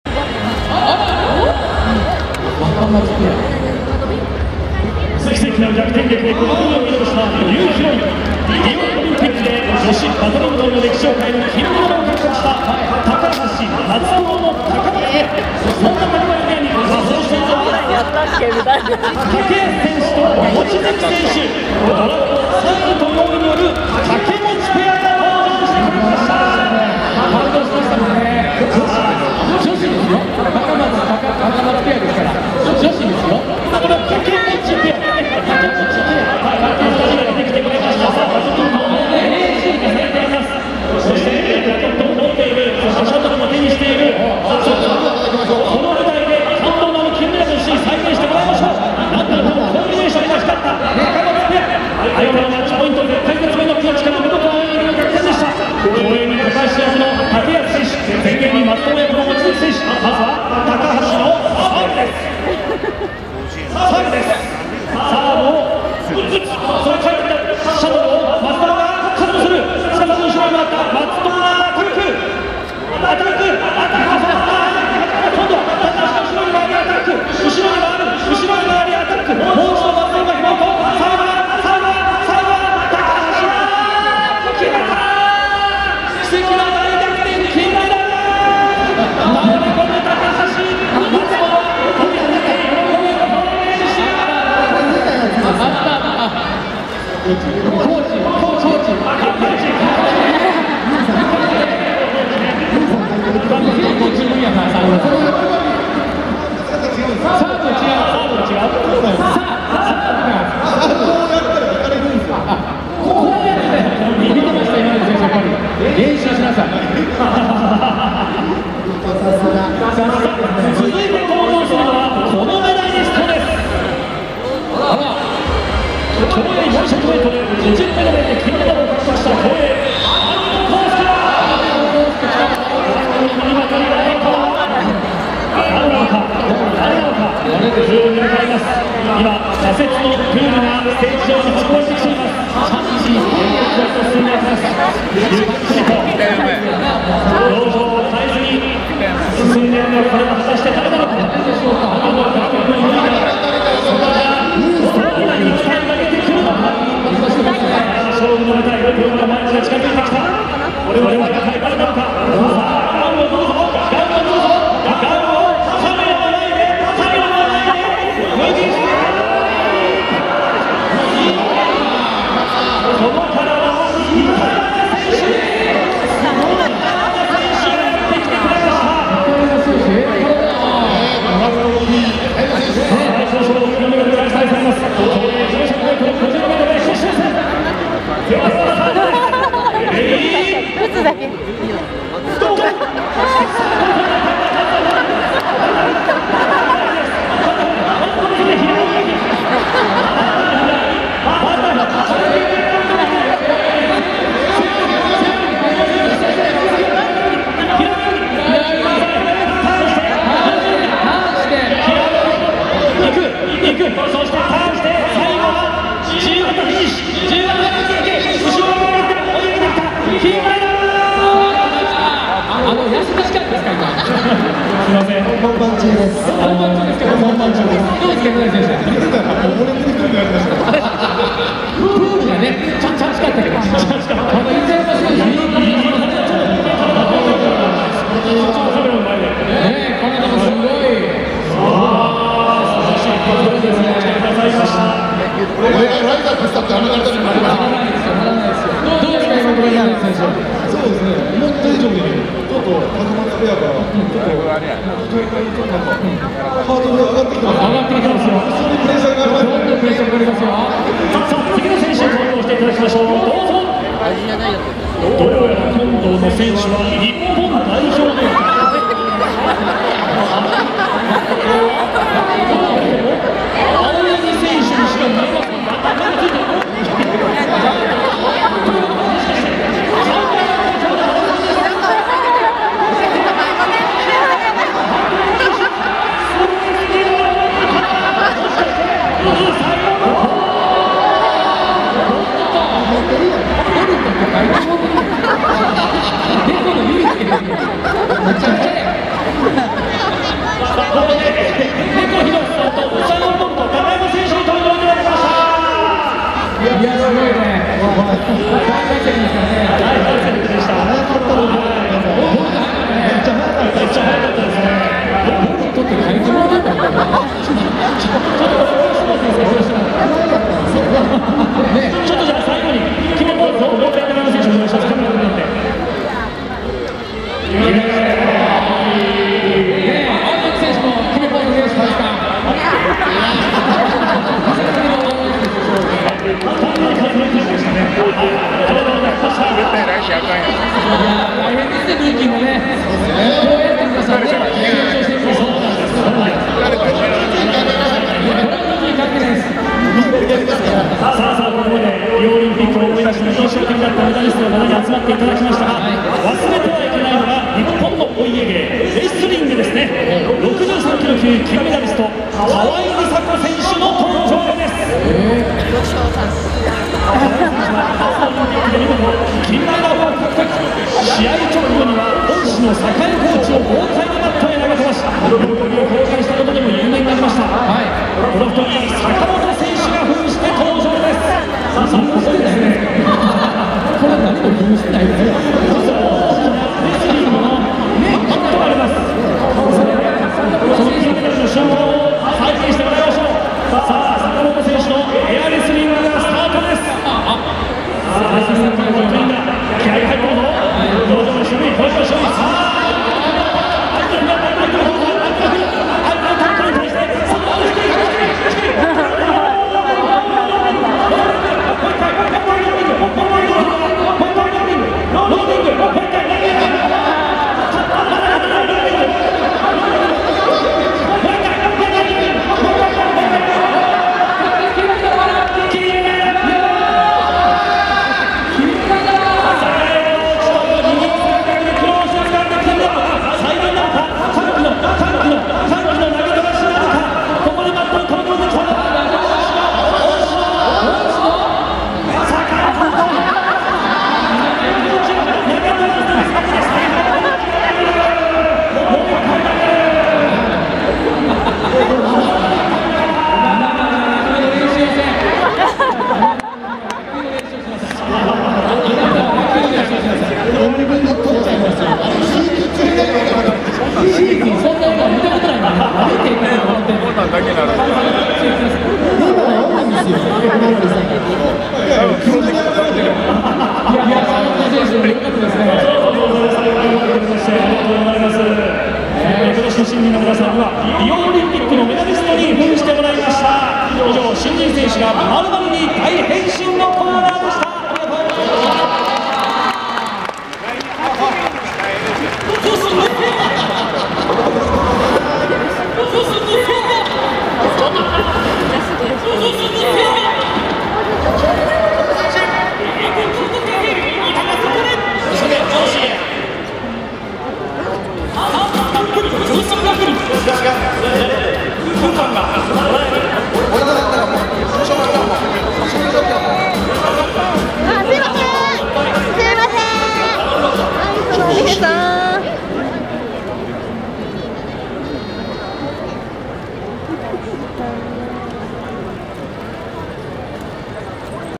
阪神タイガース2016/ファン感謝デー 新人選手が○○に変身(2016.11.19)
毎年恒例の新人選手の仮装です。
今年はオリンピックイヤーだったということで
オリンピック選手に扮しています。

最後で今成も…。 -